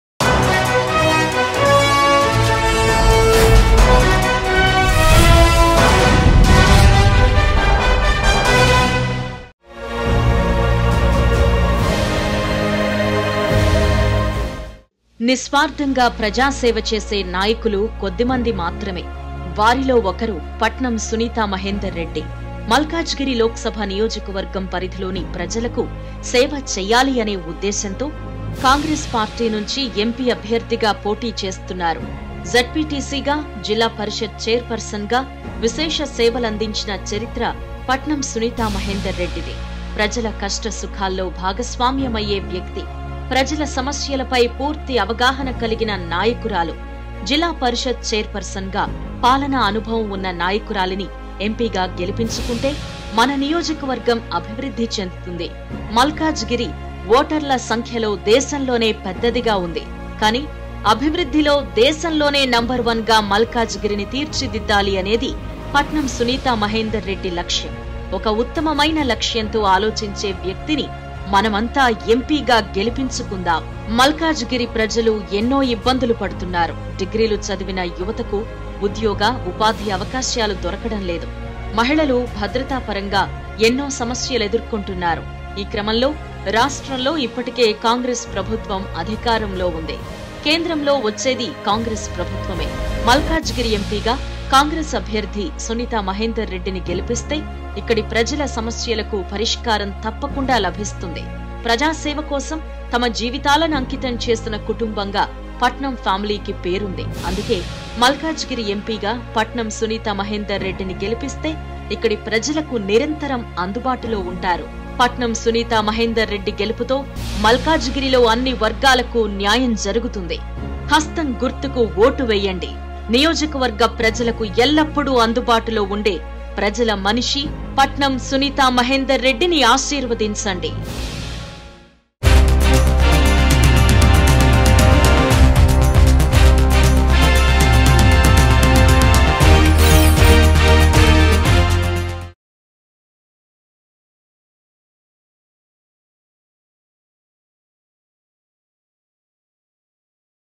Patnam Suneetha Mahender Reddy Special Story | Malkajgiri Congress MP Candidate | Mango News
Patnam Suneetha Mahender Reddy Special Story. Elections To Be Held On May 13th, 2024.